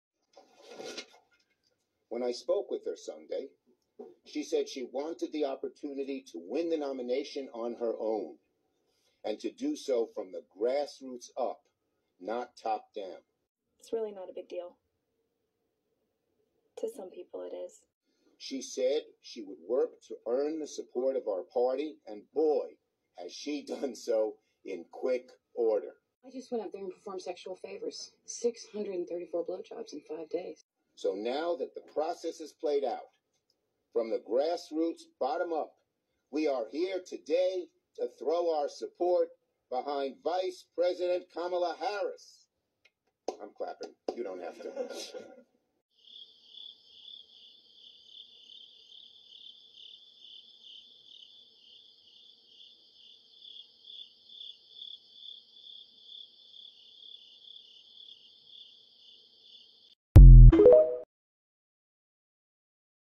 Erin Brokovich helps Kamala Harris get presidential nomination without votes.
This got reported and taken down because someone lied about it. I panicked and  removed it from YouTube. I lost a lot of views by doing that. I disputed the claim on tiktok and won. I think it's crap that someone can lie about a video they don't like and it immediately gets taken down. It's back up on tiktok and it's back in YouTube. So we'll see.